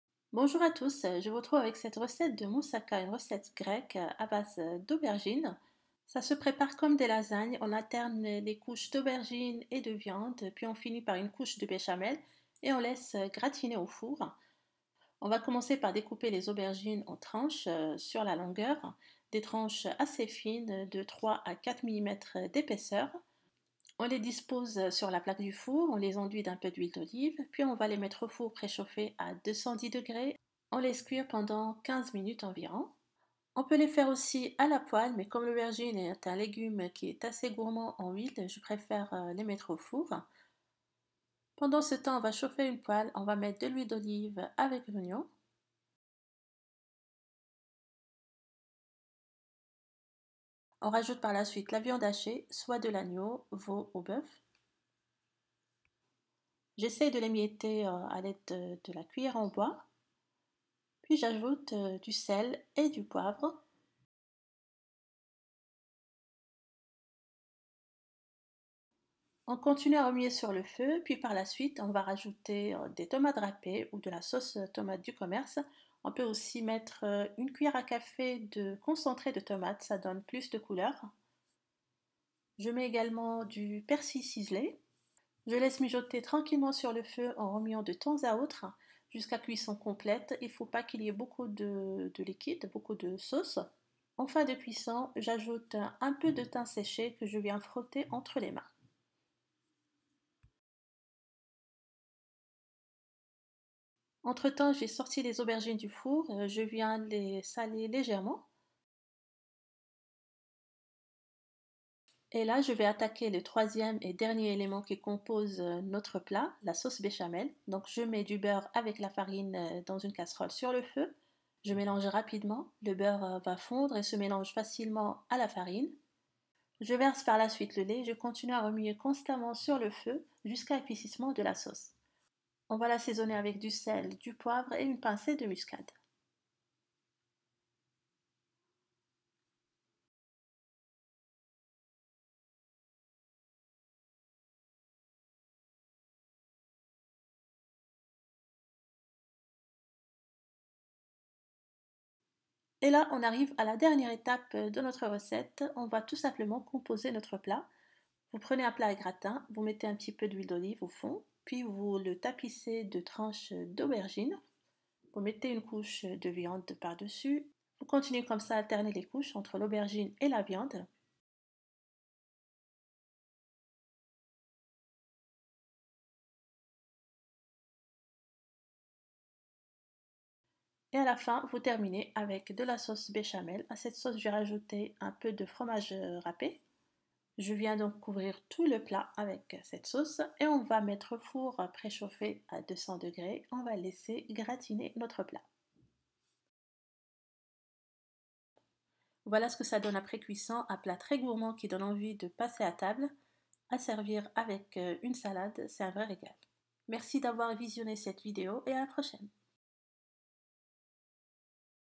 Une des meilleures façons de cuisiner l'aubergine !
Après une petite pause post ramadan je reprend les publication avec un bon moussaka, une sorte de lasagne à la grecque, un plat à base d'aubergine bien délicieux à déguster pour un repas en famille .

Ingrédients pour 6 personnes :

400 g de viande hachée
3 aubergines taille moyenne
5 tomates râpées1 gros oignon haché
1 gros oignon haché
1 gousse d'ail écrasée
2 c.s de persil ciselé
huile d'olive
sel et poivre
1 c.c de concentré de tomate
Un peu de thym séché
Sauce béchamel
1 petite poignée de gruyère râpé